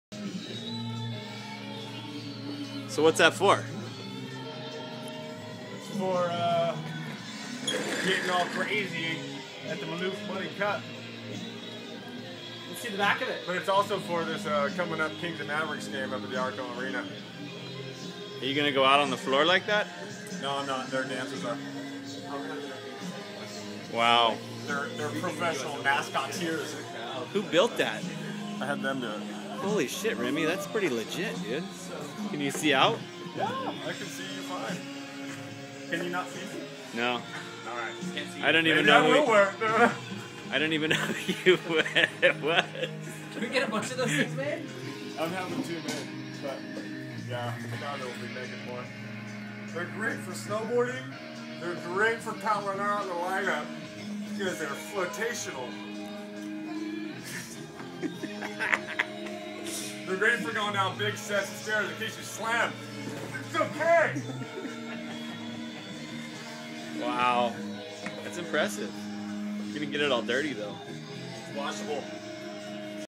Remy in stone suit
...out of his mind...mad genius Remy Stratton demonstrates the benefits of the new mascot uniform.